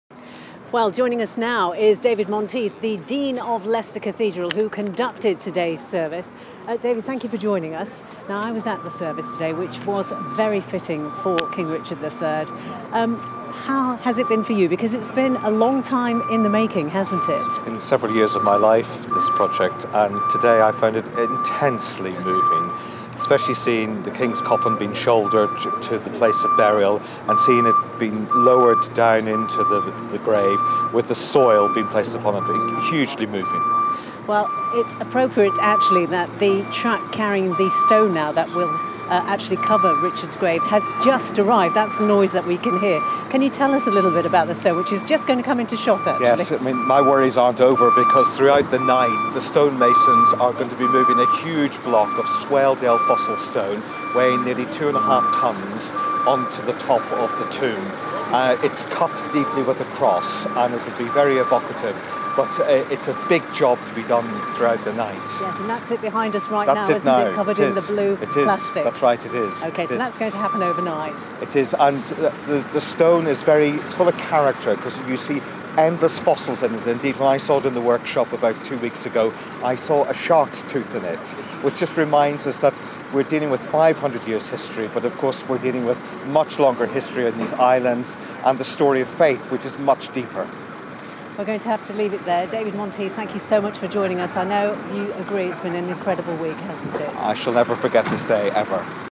The Dean of Leicester discusses the reinterment of Richard III
On Thursday 26th March 2015 King Richard III was reburied at Leicester Cathedral. In this report The Very Rev David Monteith shares his thoughts on a truly historic day.